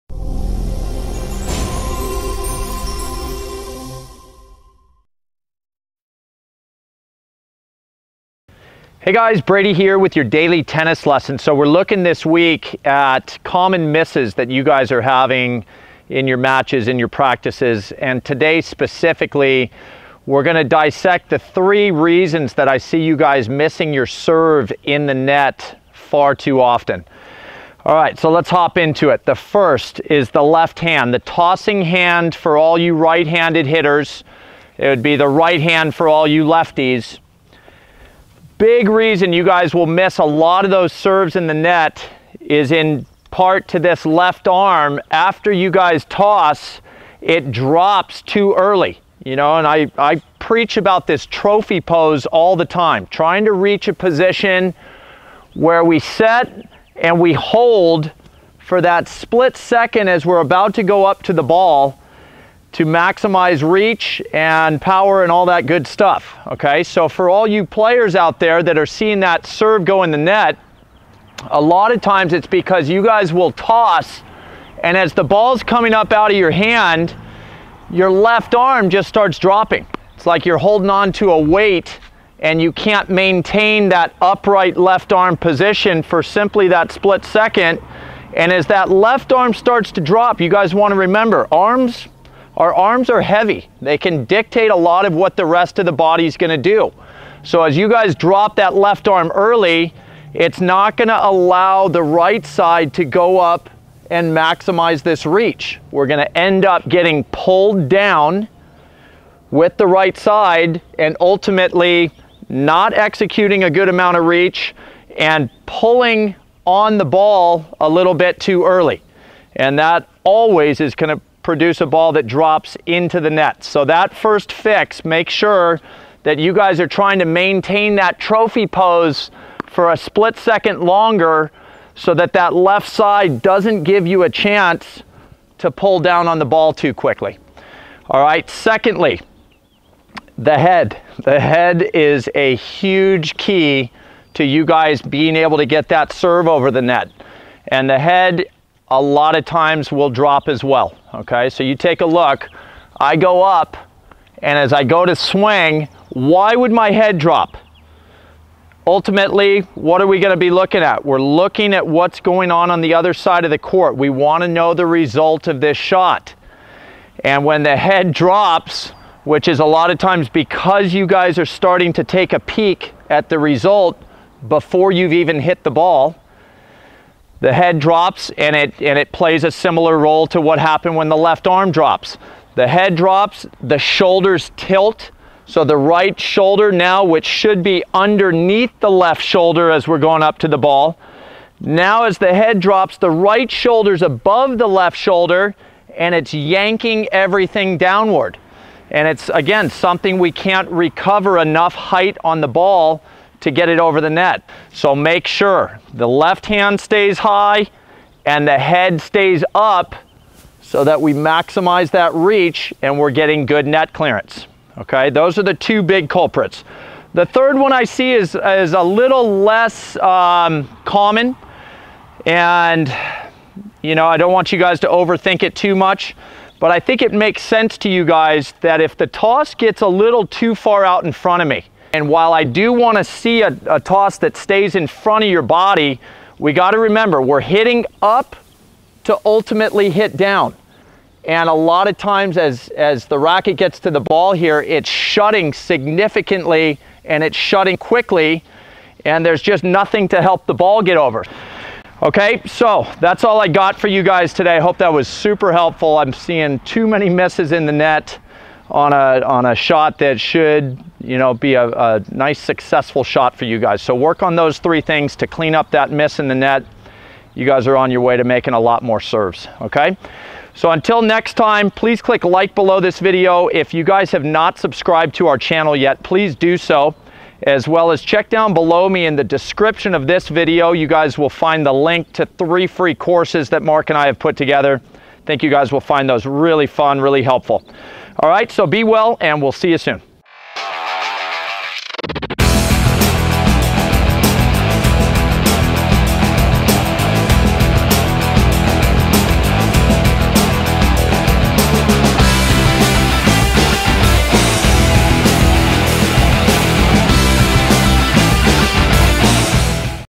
3 Reasons Your Serve Goes Into Net | COMMON MISSES
One of the most common misses in tennis is serving into the net. Why does this occur and how can you avoid it? Get your answers in today's lesson with Brady.